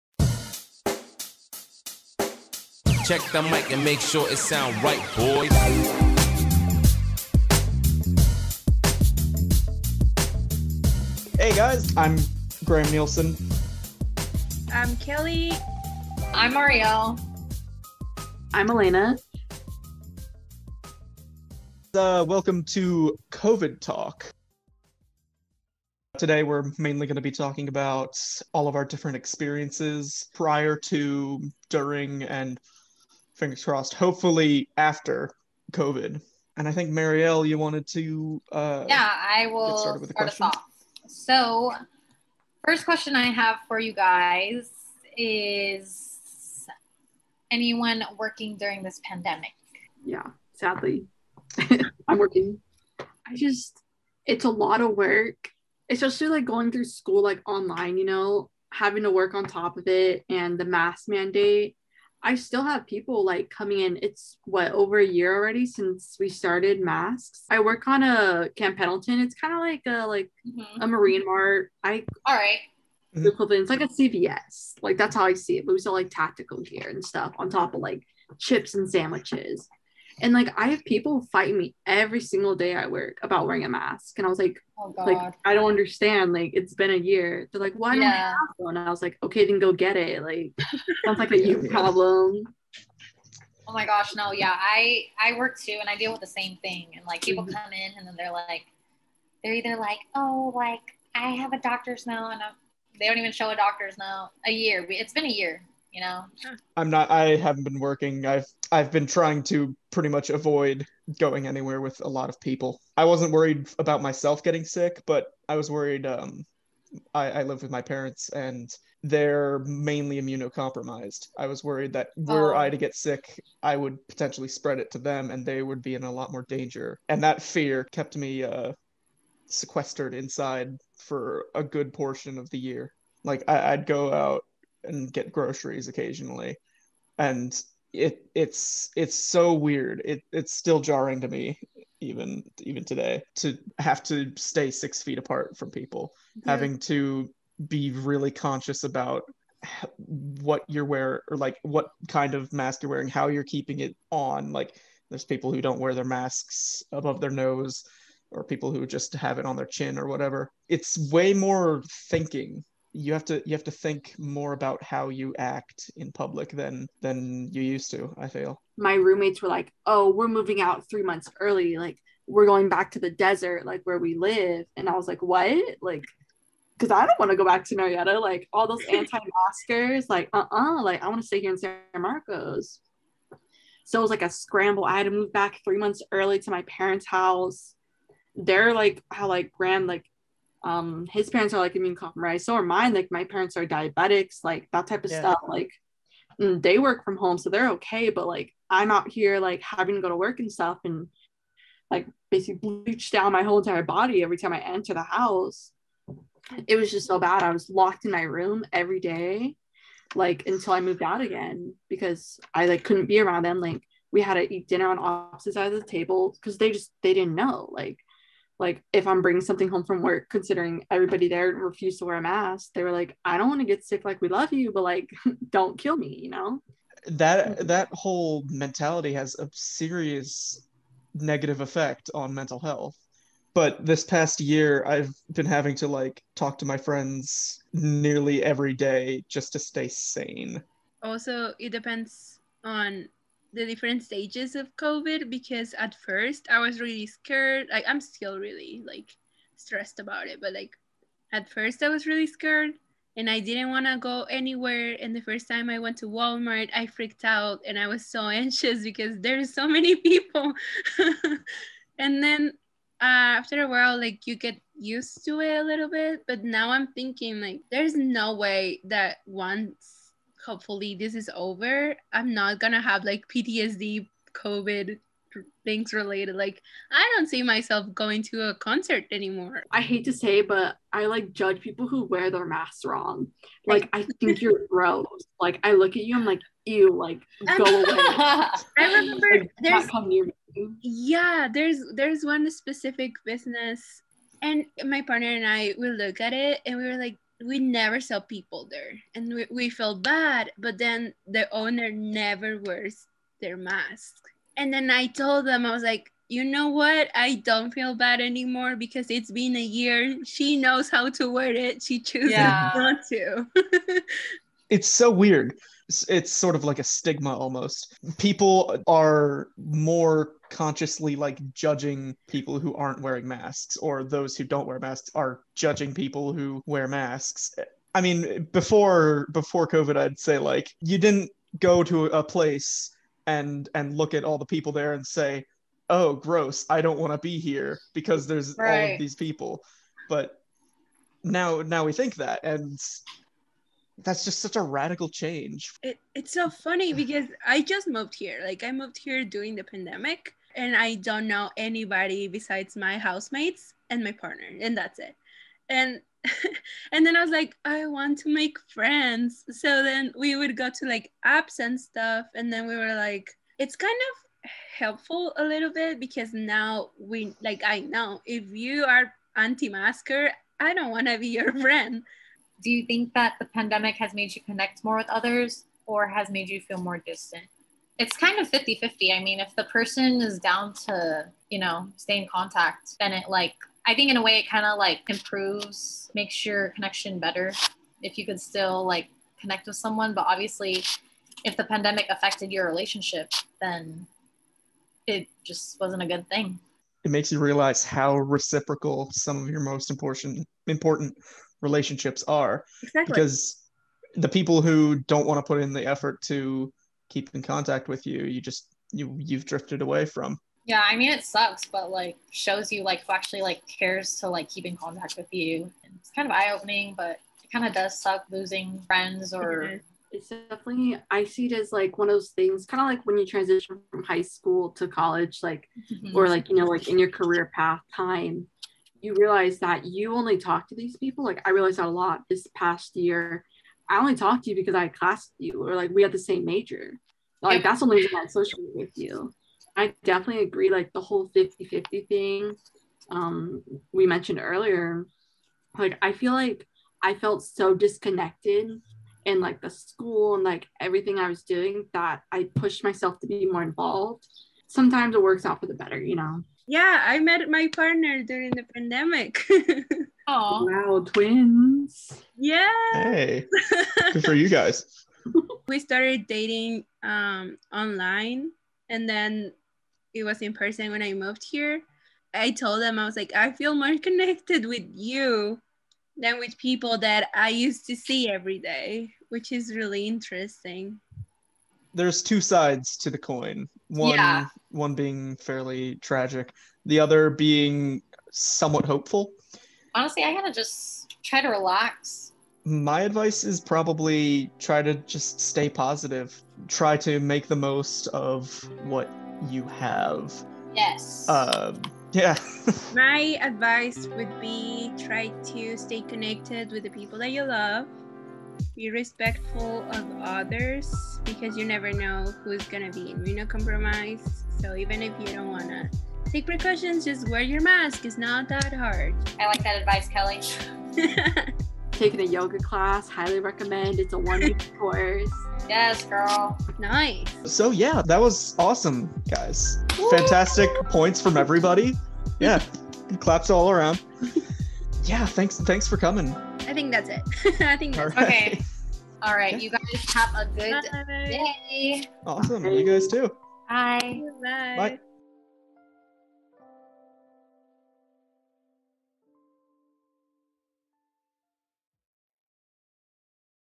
The CovidTalk Podcast: Episode 1
"CovidTalk" is a chatcast/podcast started by 4 classmates during the Covid19 pandemic. Marielle, Aliana, Kelly, and Graham got together to share their experiences as students, sons, daughters, partners, and human beings, making and questioning real connections with others and ourselves. In this episode, they talk about mental health, coping mechanisms, hobbies, anti-maskers, and more.

Sound Editing: Kelly Ibarra.